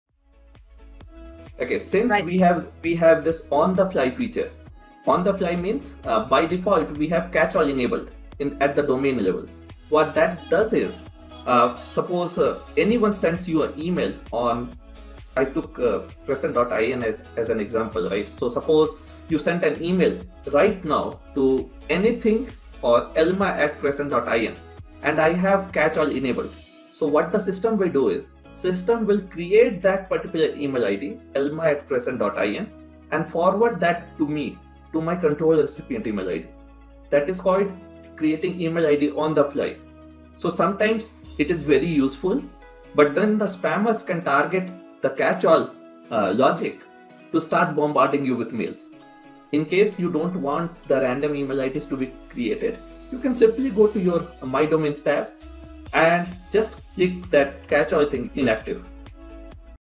Creating Email IDs on the fly | Fastest way to create Email ID | MutantMail.com
It makes your email communication more efficient by searching through your email accounts and sending the email to the correct one. It also helps you create new email ids in one click!

Problem: Imagine having 5 domains, with 5 Email IDs each (admin, support, help, legal, contact). That's 25 Email IDs. Do we want to log in to every mailbox to keep the domain persona?

Answer: Receive, Reply & Send all Emails for your Domains from your free Gmail account(or any Email account) and save on Email Hosting.

Skip the Email client setup headache!

With Mutant Mail service you can receive, send and reply all using one mailbox.